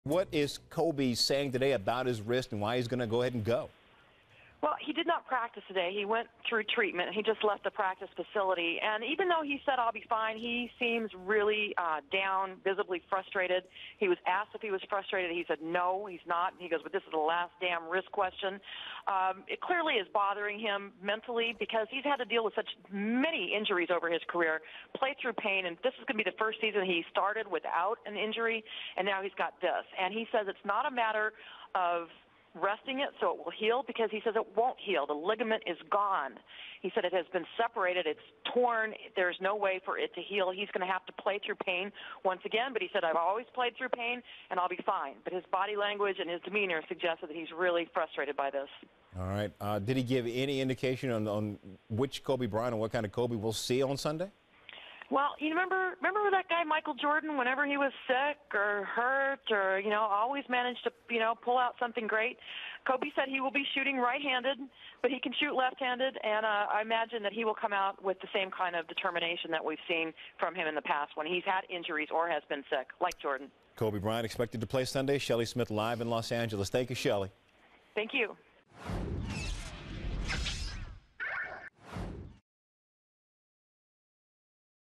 Lakers Kobe Will Play In Season Opener, Injury Update - HD Review
ESPN Shelley Smith with the latest on Kobe Bryant Injury.